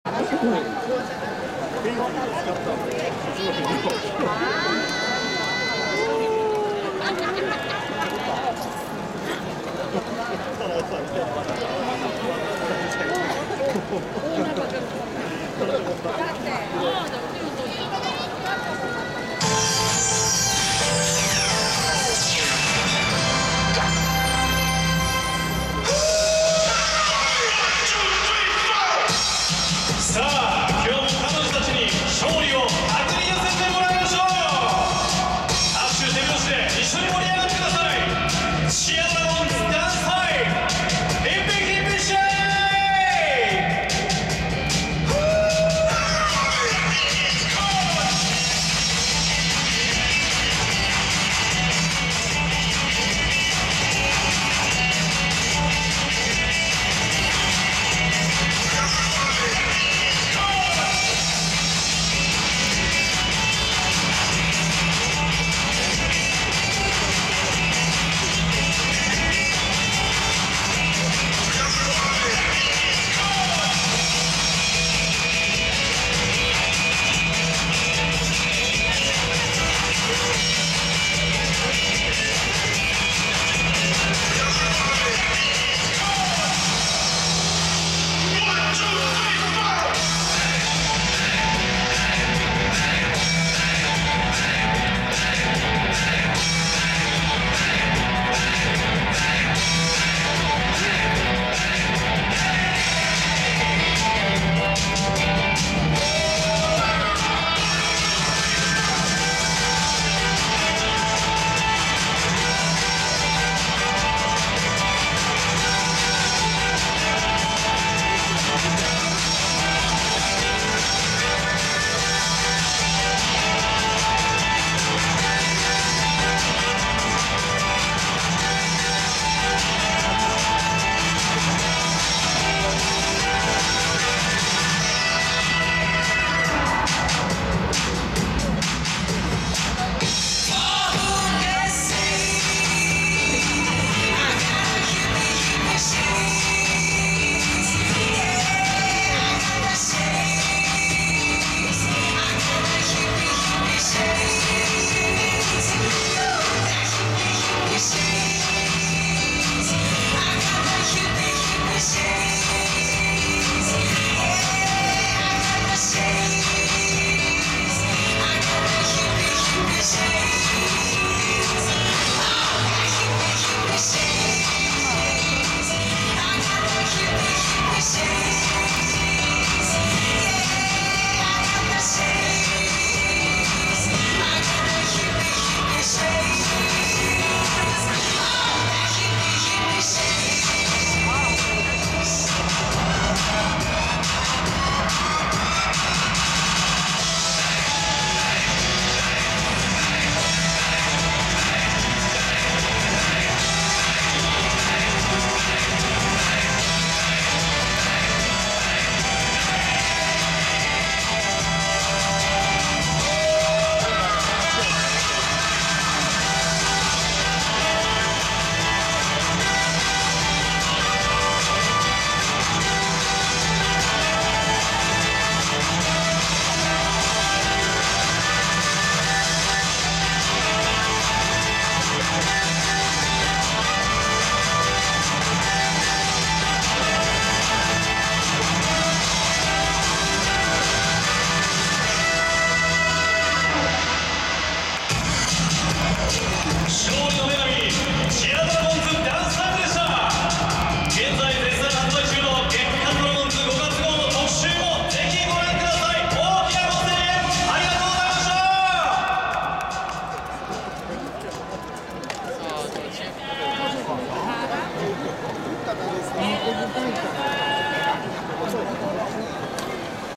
2015.04.23 チアドラ2015オープニングダンス シャオロンカメラ
いつもは「ドアラカメラ」でドアラばっかり撮ってますが、今回は「シャオロンカメラ」やってみました。